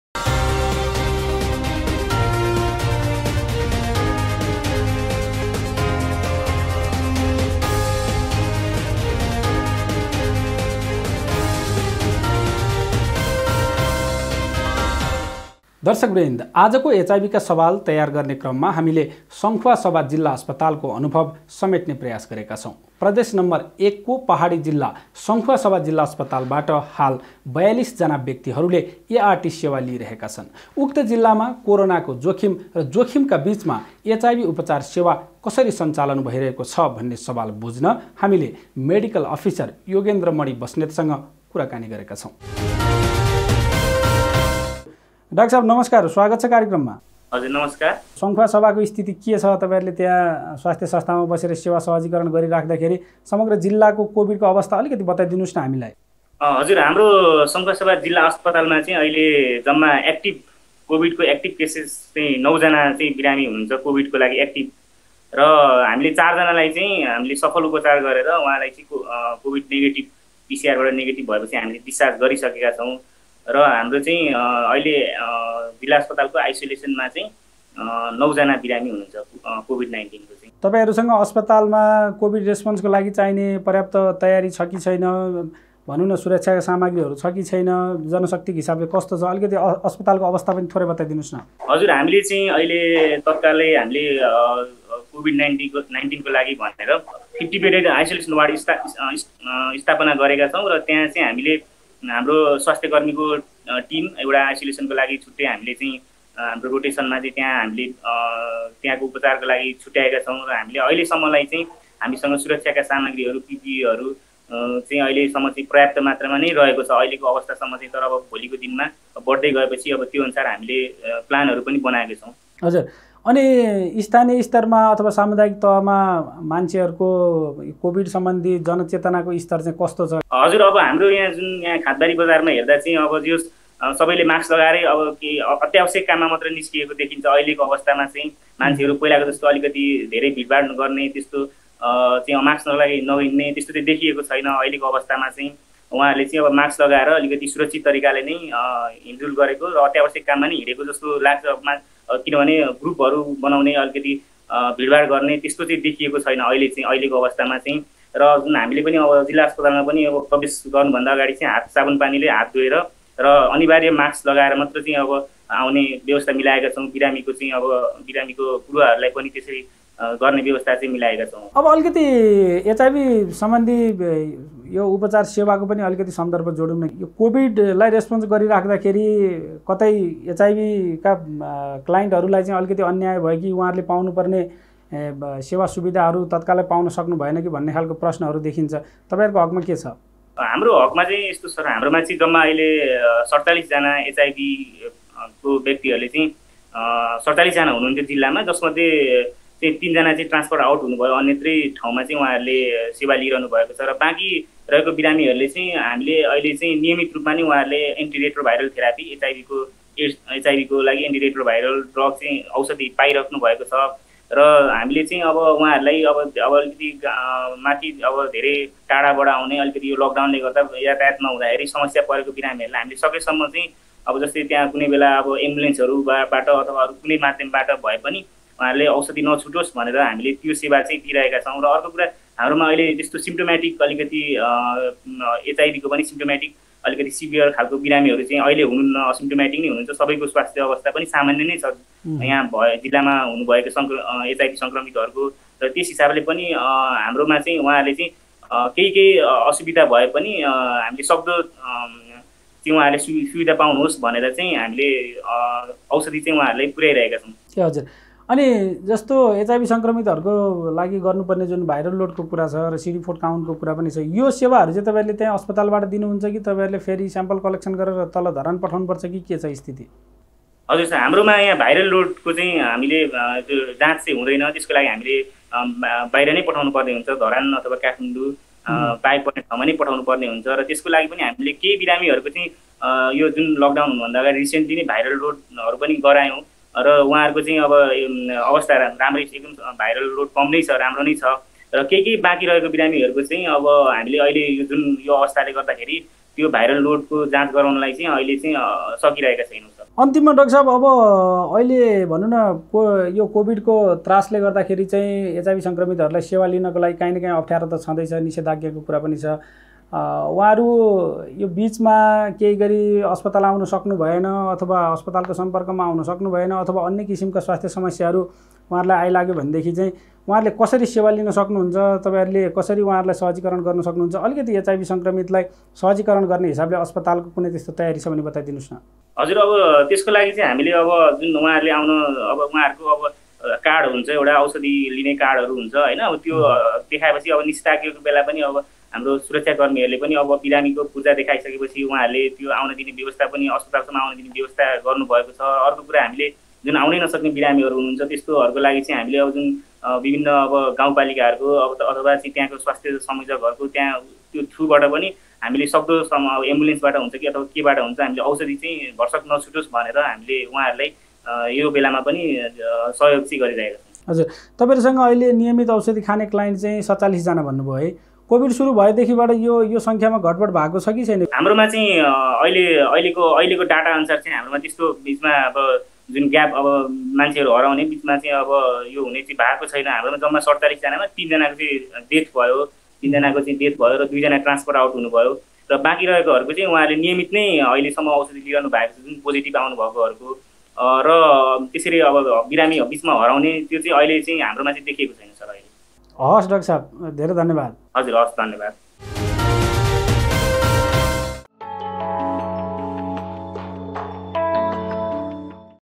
ART service in Sankhuwasabha || Episode_164
Total 42 clients are taking ART services from Sankhuwasabha district hospital of province-1. How the service is providing during the risk of COVID? The episode covers the situation of Sankhuwasabha.

Bites
∙ Dr. Yogendramani Basnet, Medical Officer, Sankhuwasabha Hospital.

Broadcast Date: September 05, 2020